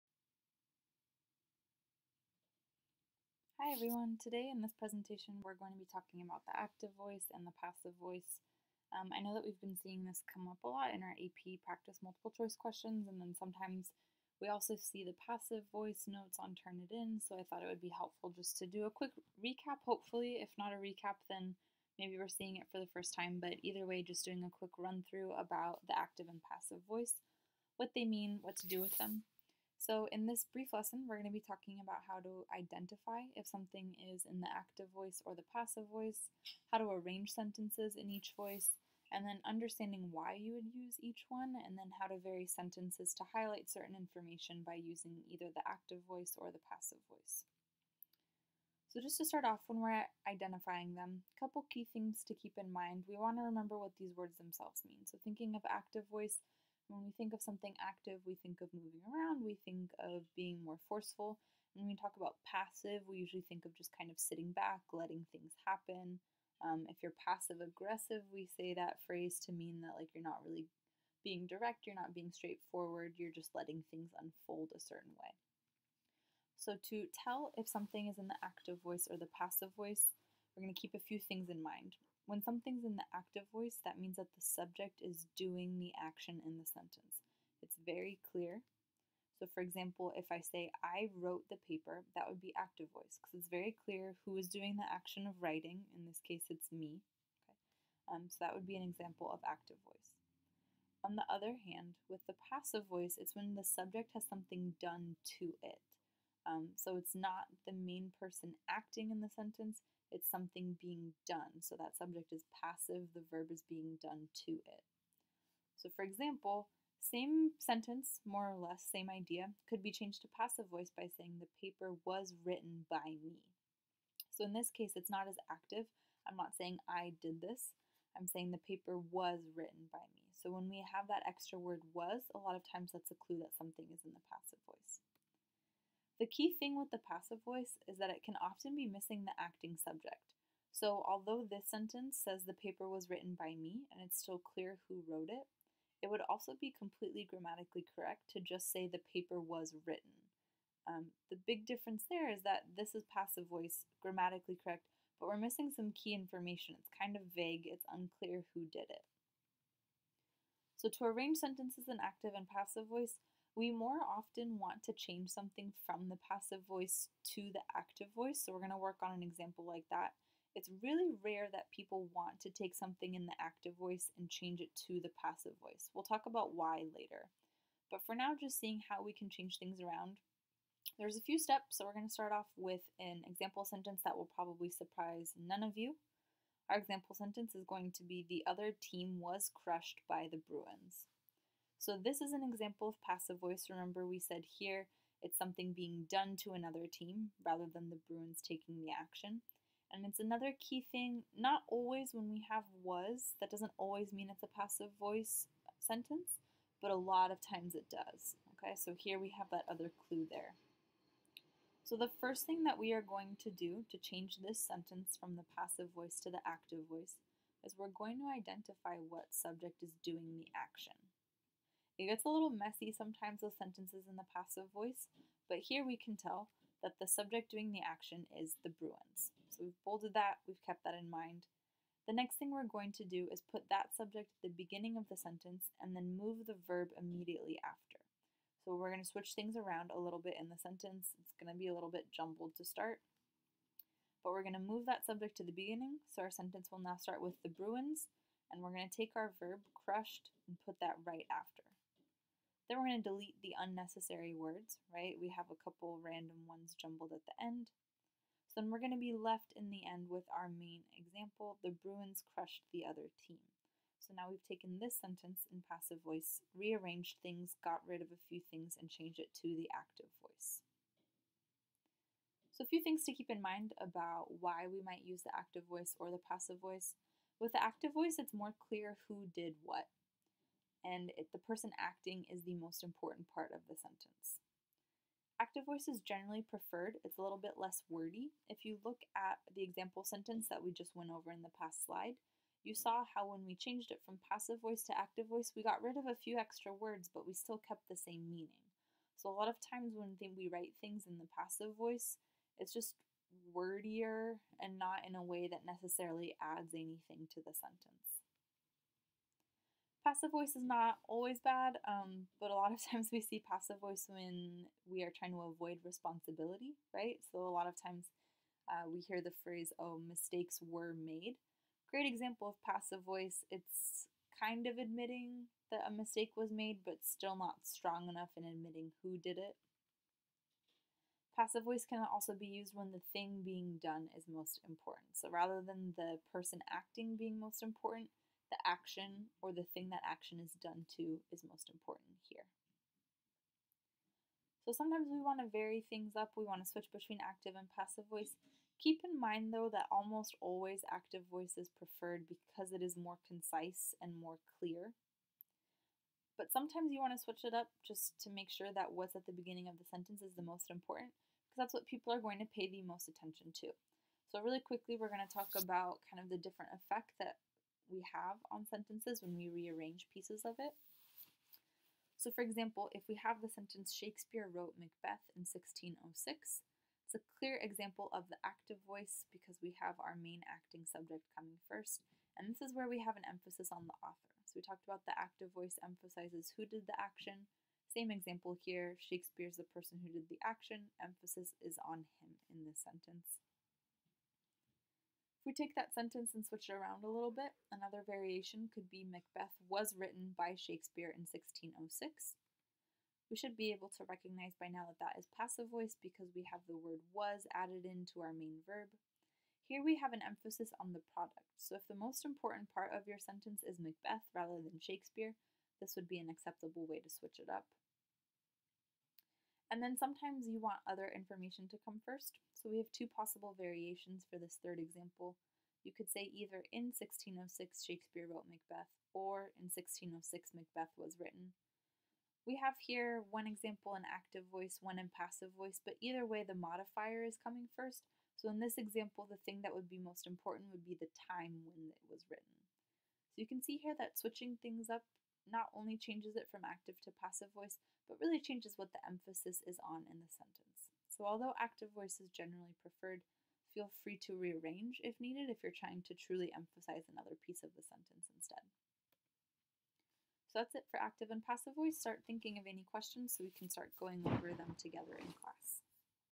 Active vs. Passive Voice
-Identifying active and passive voice
-Arranging sentences in active and passive voice
-Understanding the use of active and passive voice
-Varying sentences by using active or passive voice